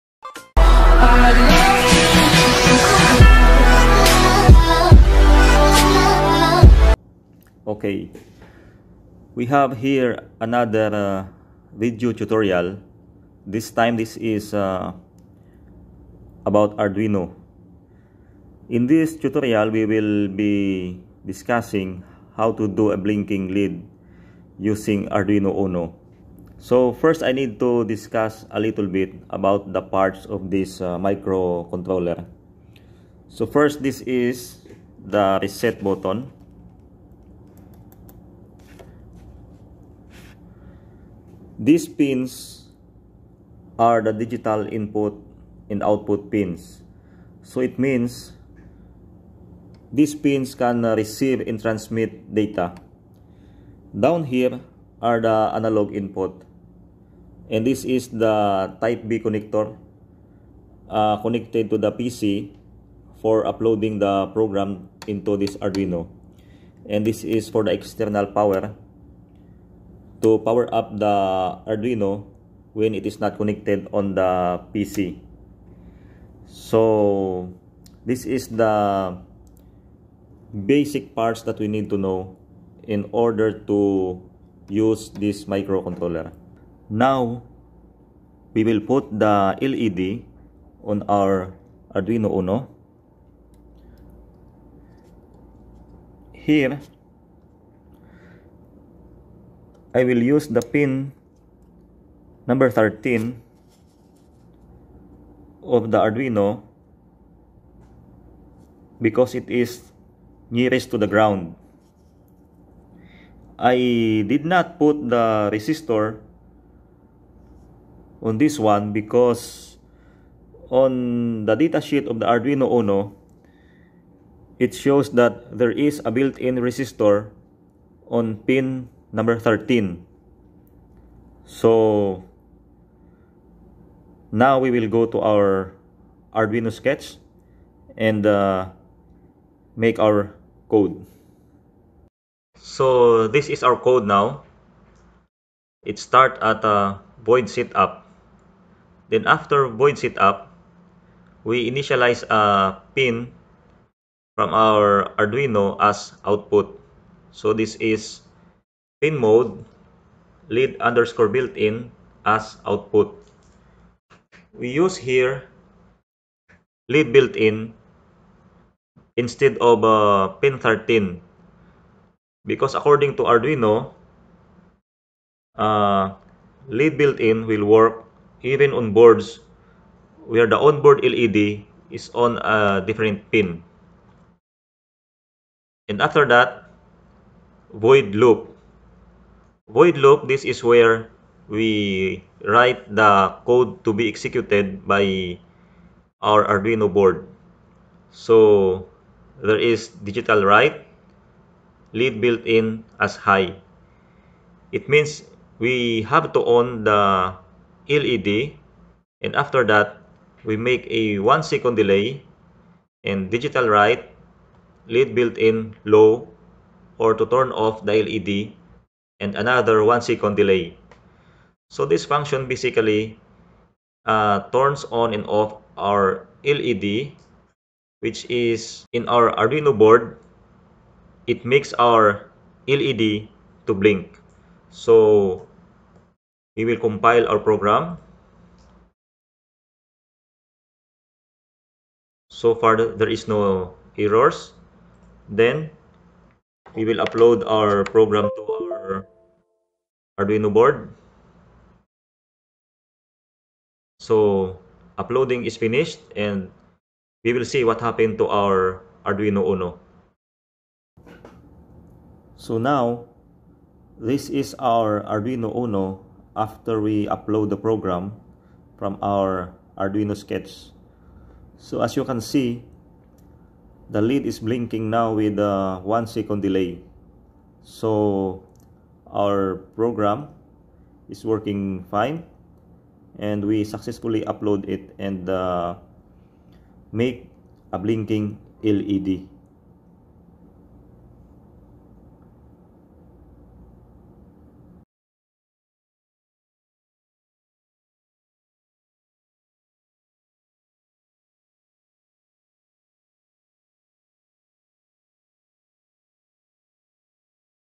Blinking LED, Arduino for Beginners
Arduino tutorial for beginners. This tutorial shows how to make a blinking LED using Arduino Uno.